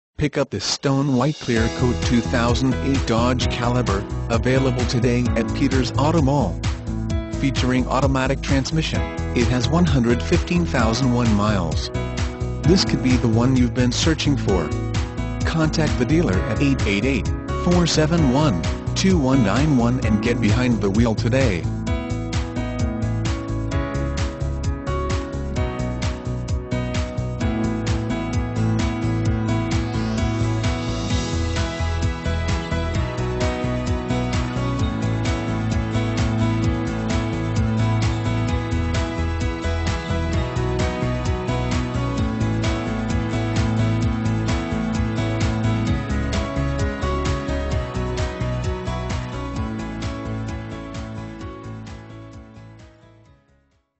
2008 Dodge Caliber High Point North Carolina 22178A
2008 Dodge Caliber SE - $7850

Pick up this Stone White Clear Coat 2008 Dodge Caliber, available today at Peters Auto Mall. Featuring Automatic transmission, it has 115001 miles. This could be the one you've been searching for.